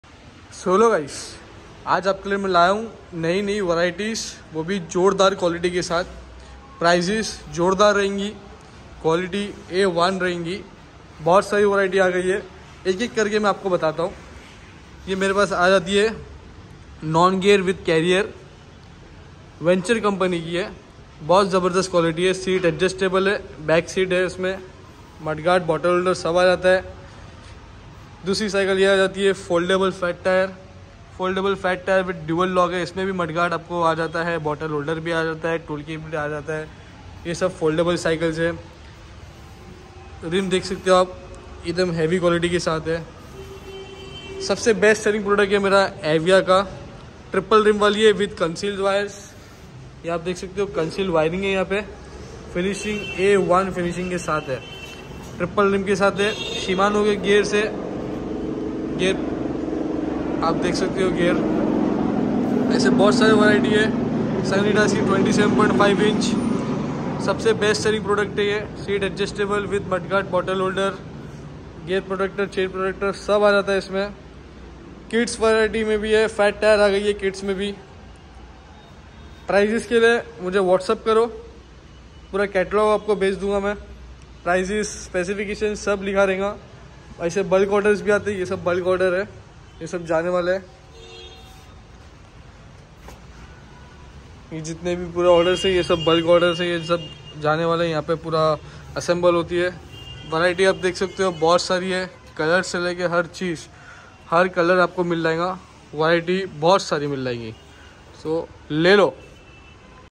Wholesale bicycle shop in santacruz west Cod available all over india
Karwat enterprises deals with all imported bicycles at wholesale price ,Delivery available all over india with cod facility
All varities available from mtb, roadbike ,kidscycle ,fat bike

Address - Dheeraj heritage, 1st floor, shop no 111, opp milan subway ,above lenskart Santacruz west-Mumbai -400054